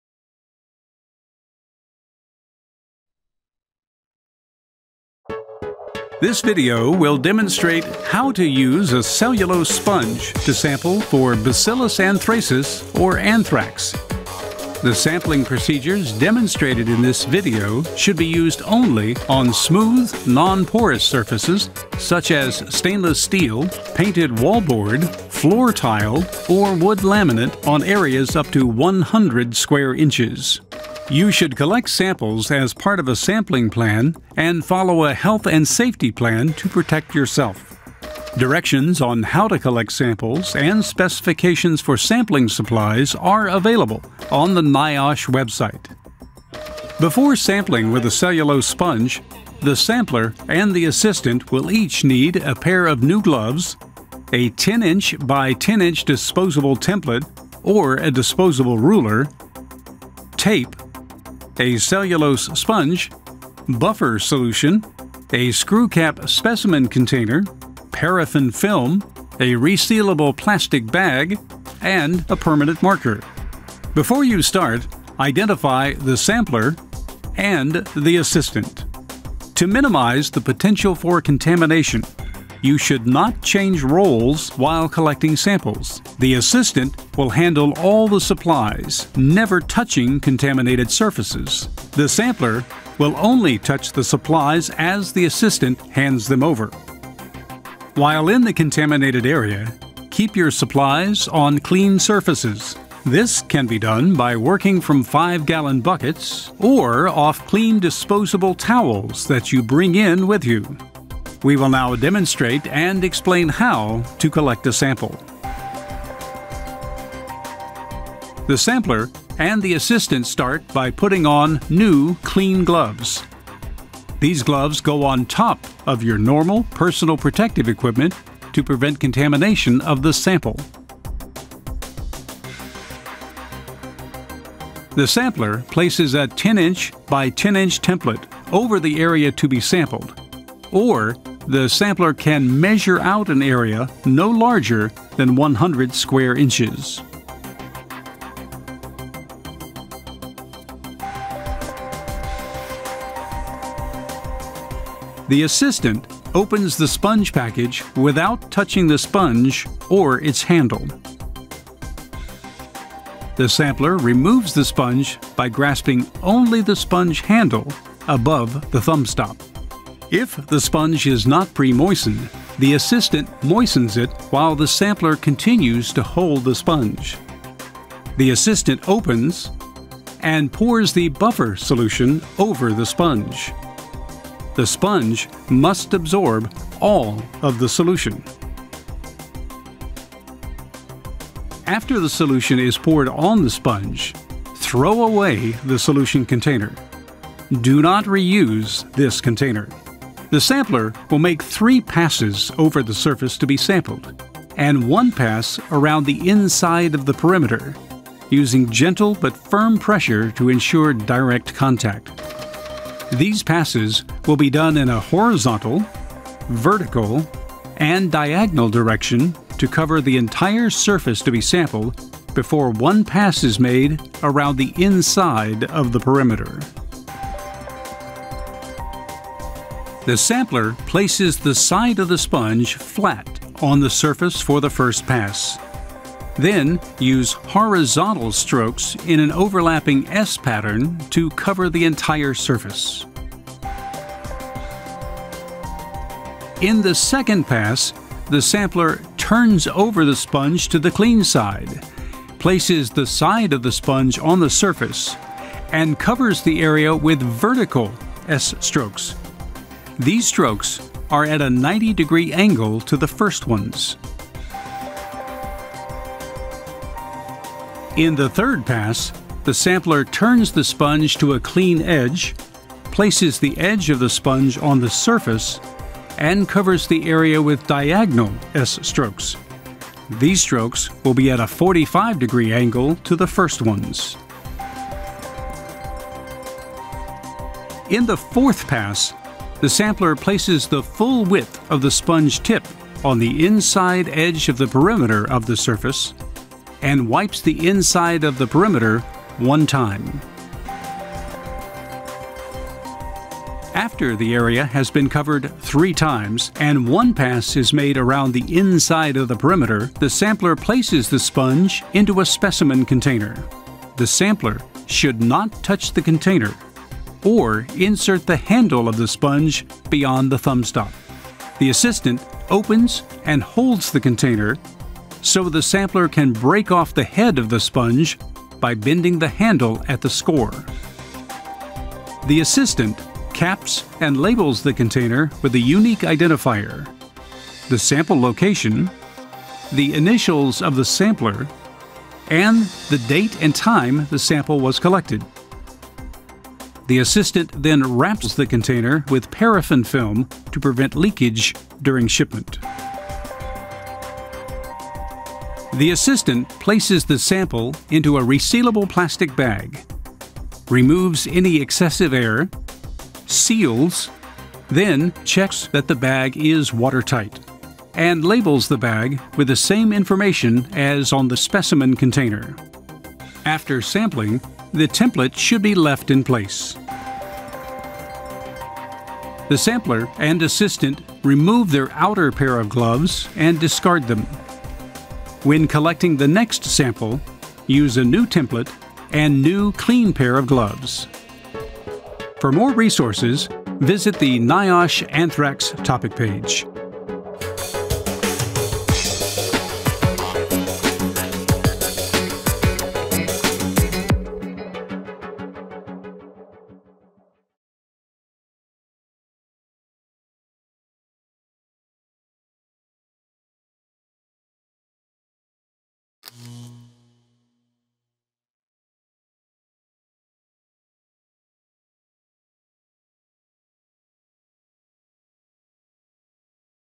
Anthrax surface sampling: How to sample with cellulose sponge on nonporous surfaces
This training provides instructions for the collection of environmental Bacillus anthracis spores, or anthrax, from nonporous surfaces using cellulose sponges. The collection method demonstrated in this video is based on the CDC/NIOSH surface sampling procedures for Bacillus anthracis spores from smooth, non-porous surfaces. This method is available on the NIOSH